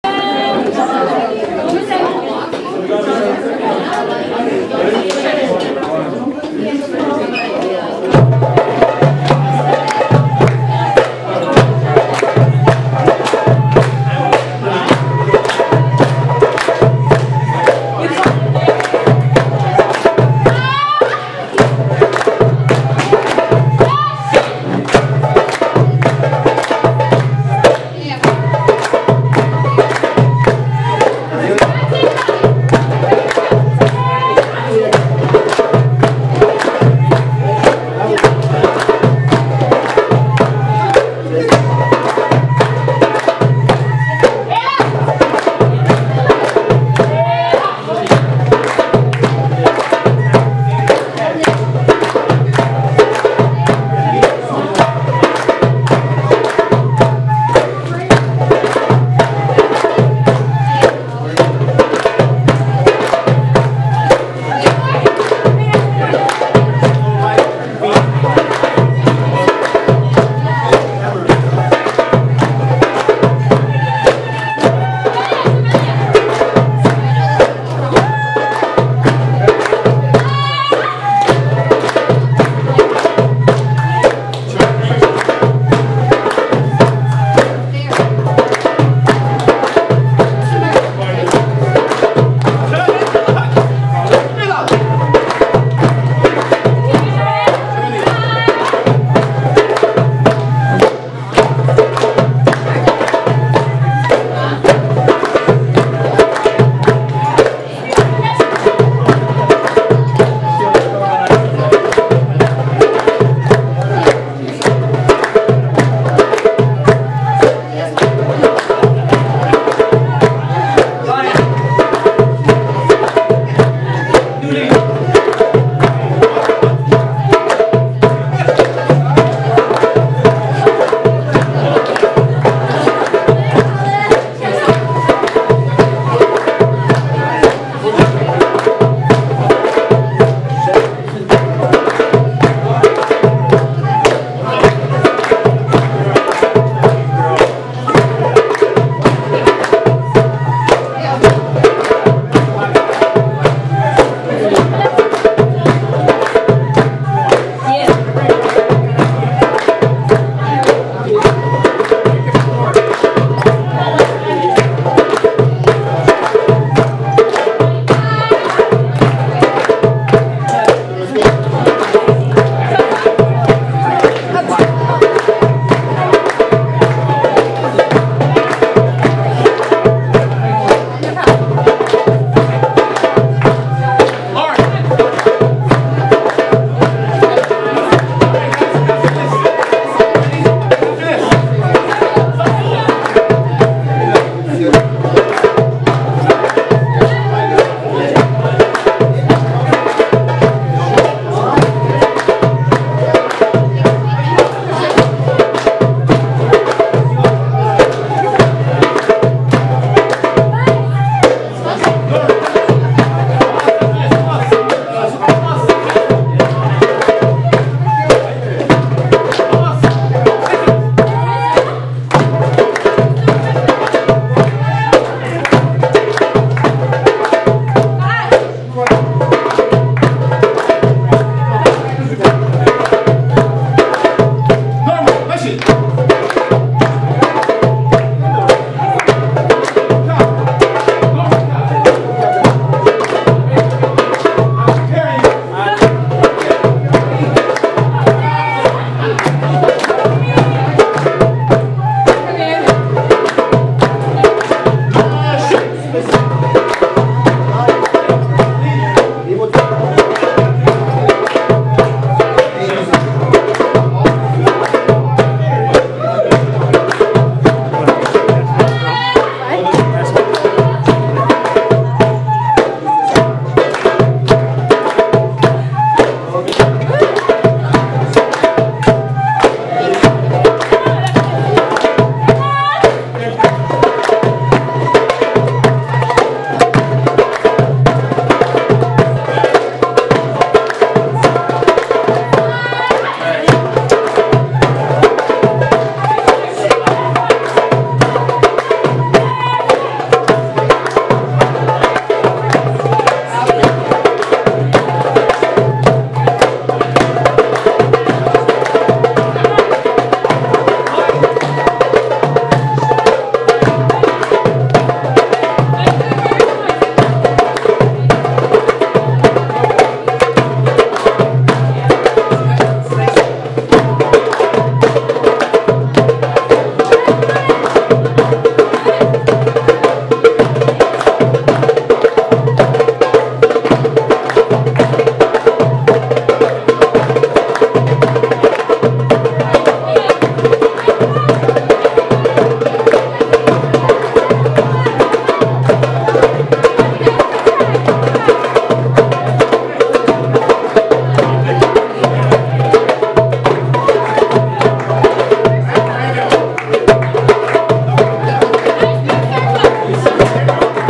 Shamaliya.mpg
Al-Haneen dabke performance at Ali Baba restaurant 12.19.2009